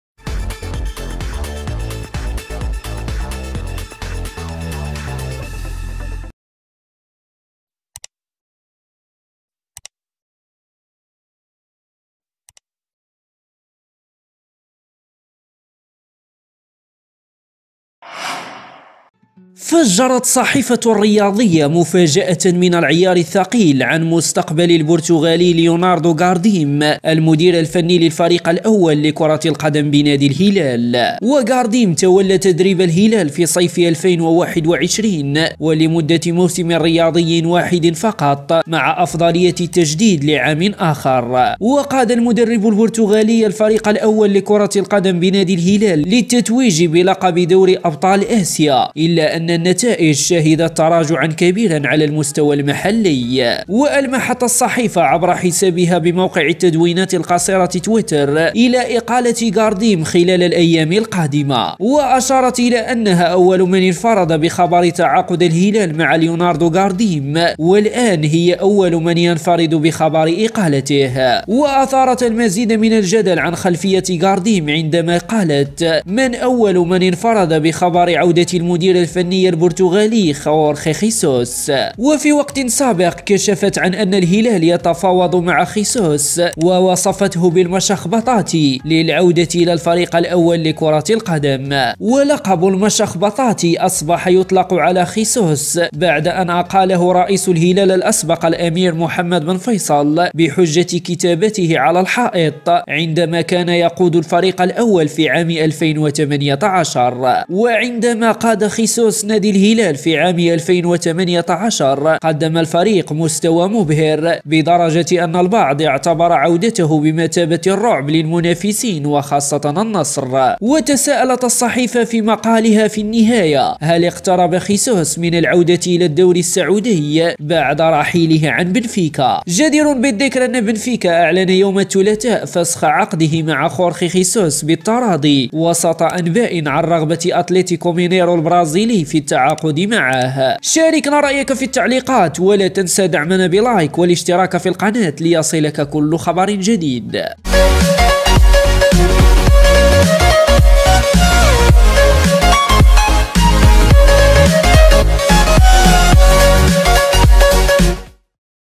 بن نافل يصالح جماهير الهلال بإقالة جارديم والتعاقد مع الثعلب خورخي خيسوس في صفقة ترعب النصر
▬▬ஜ۩سوكر_نيوز ۩ஜ▬▬
2- أرجو منك تفعيل الجرس 🔔 لتكون اول من يعلم بالاخبار
3- ولا تنسى اللاااايك 👍 سيفيدني كثيراآ ❤
▬▬ஜ۩سوكر_نيوز ۩ஜ▬▬